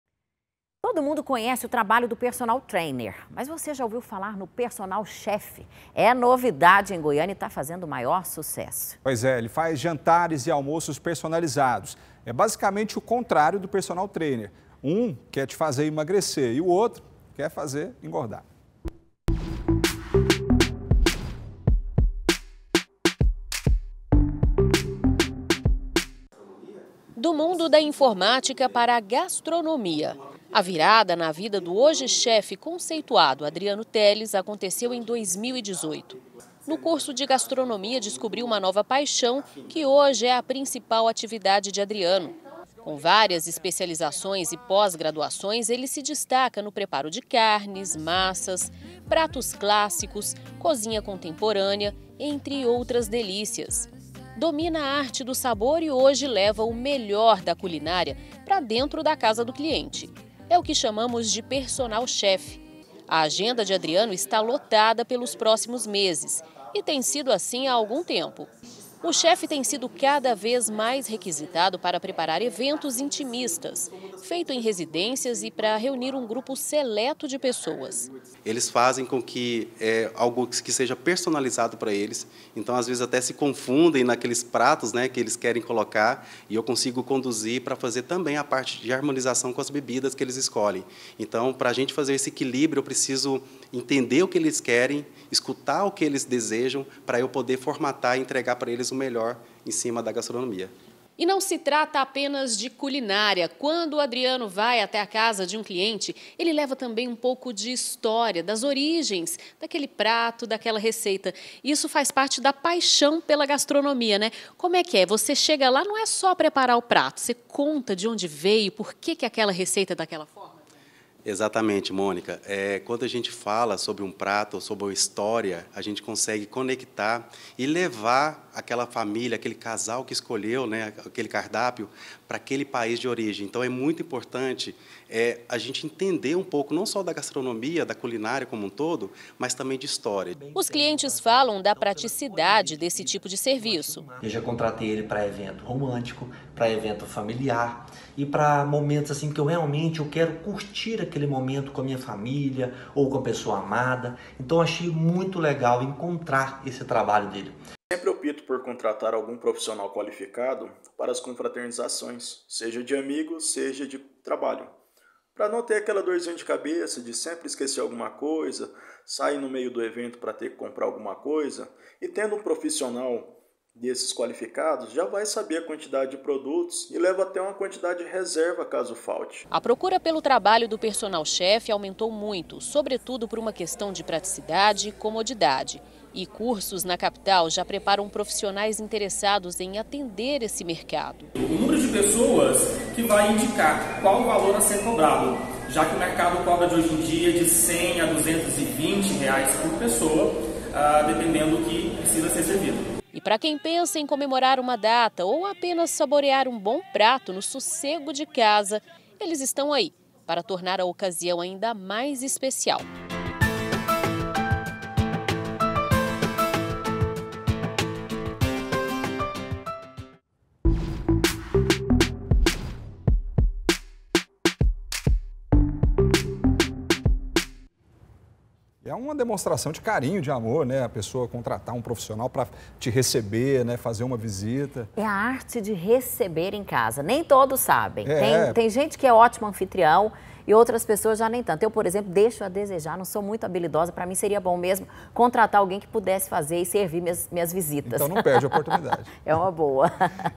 MERCADO EM MOVIMENTO: AUMENTA PROCURA PELO TRABALHO DO "PERSONAL CHEF"
TODO MUNDO CONHECE O TRABALHO DO PERSONAL TRAINER. MAS VOCÊ JÁ OUVIU FALAR NO "PERSONAL CHEF"? É NOVIDADE EM GOIÂNIA E TÁ FAZENDO O MAIOR SUCESSO.